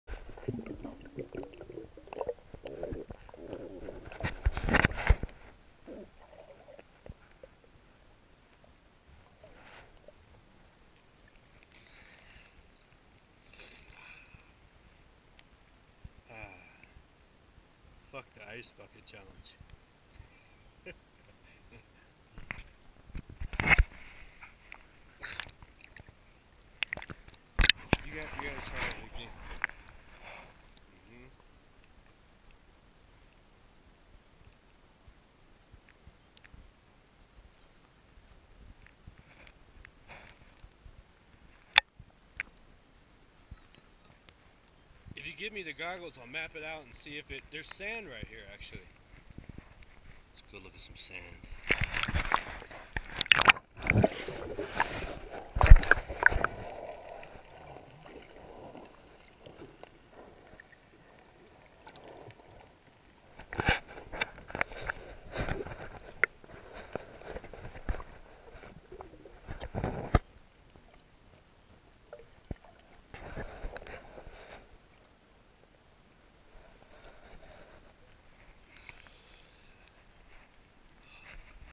Colchuck Lake: Leavenworth
Colchuck Lake
Central Cascades -- Leavenworth Area
8.0 miles, round trip
ELEVATION
Gain: 2280 ft.
Highest Point: 5580 ft.